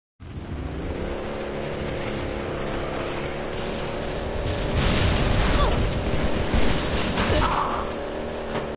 3Xmongoose kill
GamerTag: Buckster99 / Film Name: 3Xmongoose kill
Description: A TRIPLE Mongoose Mowdown! Has to be seen to be believed!
Slot: 21